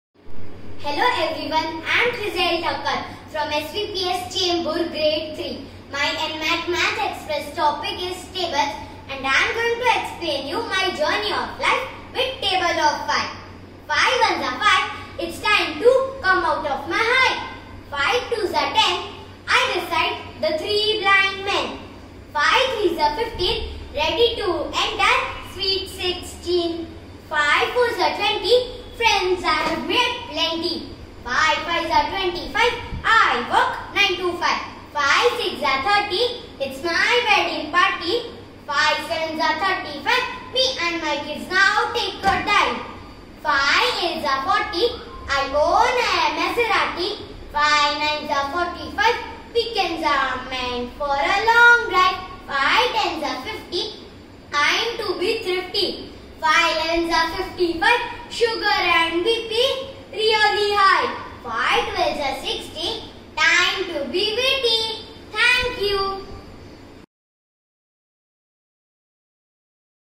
KHRISZYLLE THAKKAR (3rd Standard) NMAC Math Express 2019-20 Grand Finale by Walnut Excellence
MATHEMATICS is not about Numbers! Equations! Computations! Or Algorithms! It is about UNDERSTANDING‼️

ONLINE NMAC Maths EXPRESS Grand Finale Round 2019-20 is the unique feature of National Maths & Arithmetic Competition.  Top 43 winners of Diamond Cadre are eligible for NMAC Maths EXPRESS Grand Finale. They are selected based on their PAN Level performance in NMAC Round examination.

Selection of NMAC Grand Finale Math Express Winner are based on the basis of Virtual Videos shares by the NMAC Diamond Cadre Participant.  Students have provided the VIDEO on specific Maths Topic given to them, relevant to their standards, with the use of Props, Drama and Intellectual Thinking with creativity.

Assessment is based on five parameters, i.e. (1) Student Expression, (2) Speed, (3) Topic knowledge (4) Innovation and (5) Creativity which carries 15 marks each and another parameters are Public Opinion on Social Media Platforms.

NMAC Math Express is initiated by Walnut Excellence Education